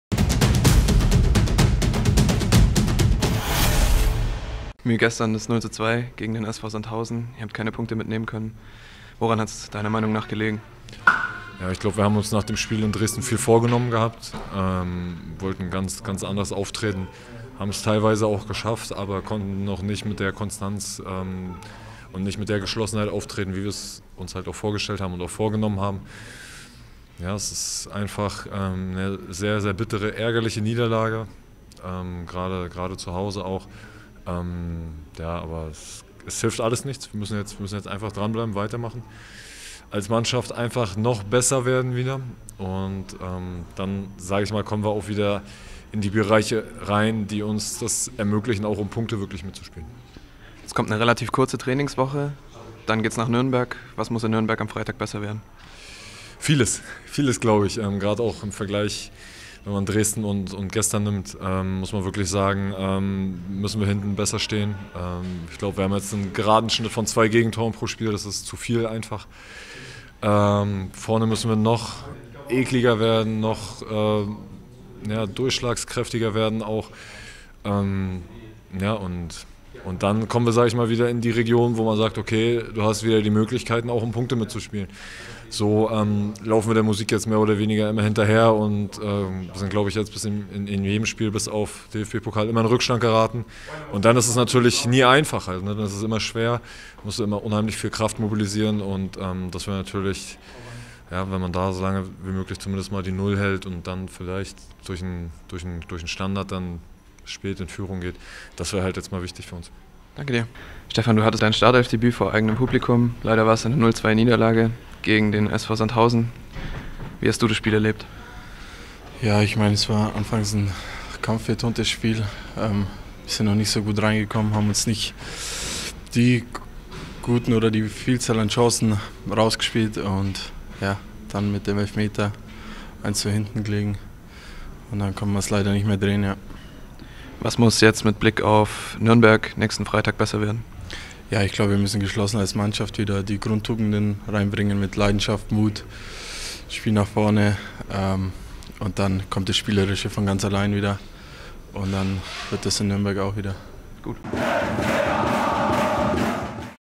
Stimmen nach dem Heimspiel gegen Sandhausen
Wir haben mit unserem Torhüter Kevin Müller und dem Stratelfdebütanten Stefan Schimmer über die 0:2-Niederlage gegen den SV Sandhausen gesprochen.